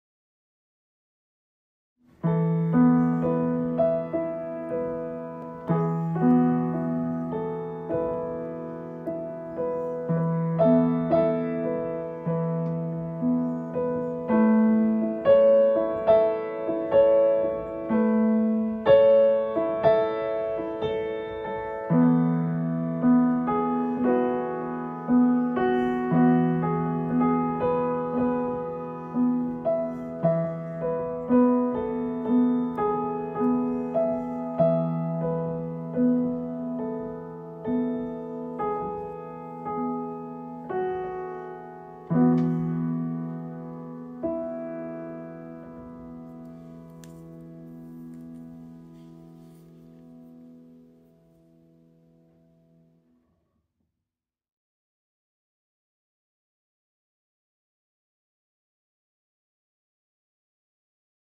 The Trees by Manor House Gate, Finsbury Park, London, my painting process with piano solo
Fleeting colours of morning light spark everything in different ways, the dark fir gets animated & every shade of green glows from the scene.